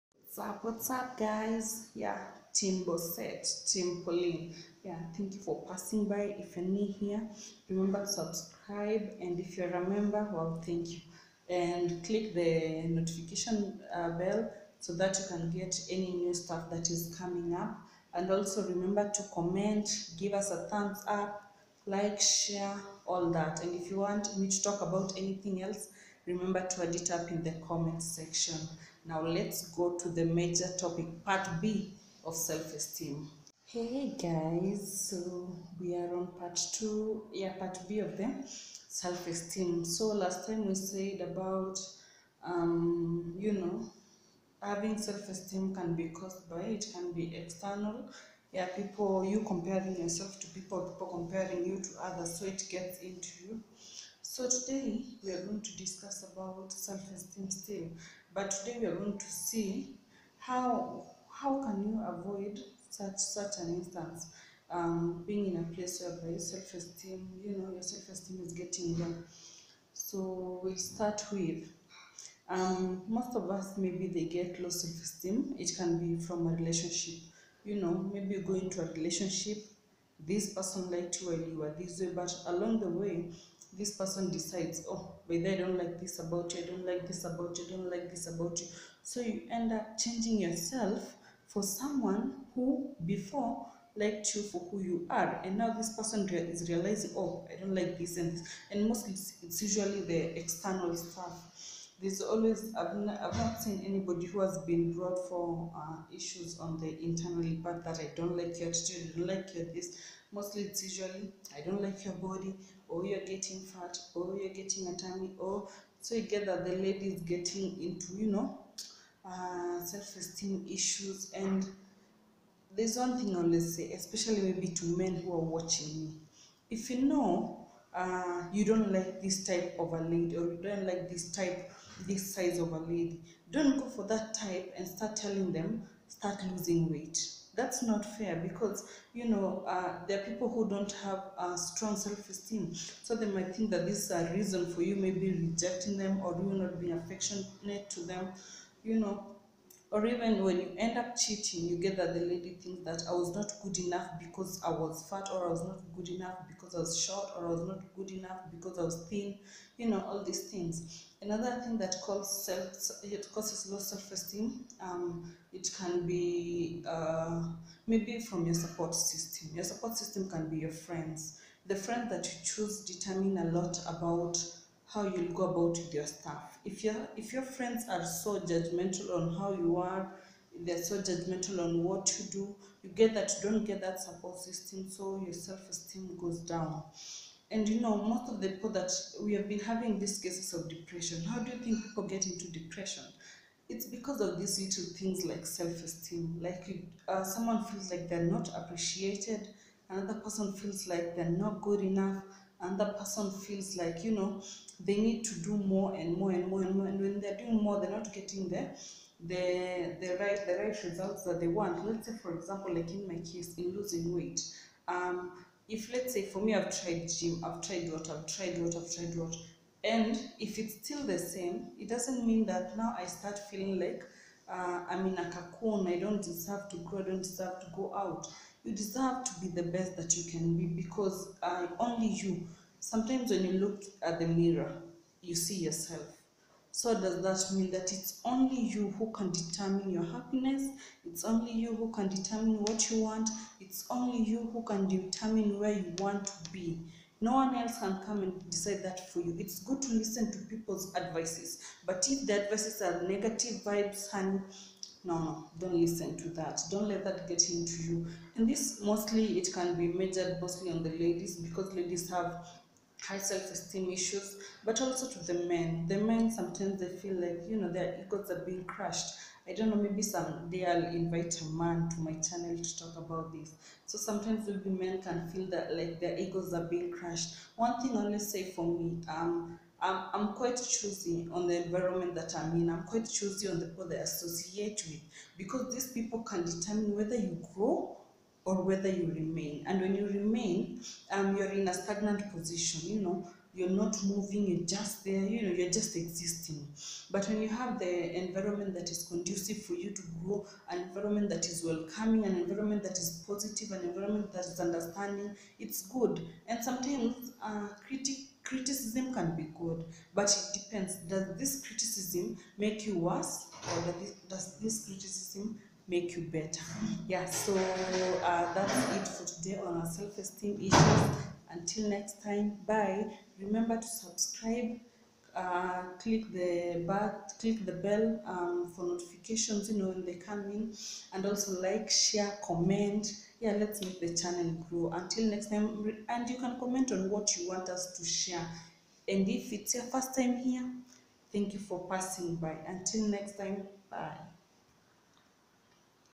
HOW TO OVERCOME LOW SELF ESTEEM (PART B)
Low self esteem can lead to depression let's all come together and cheer each other up to boost their confidence and high self esteem.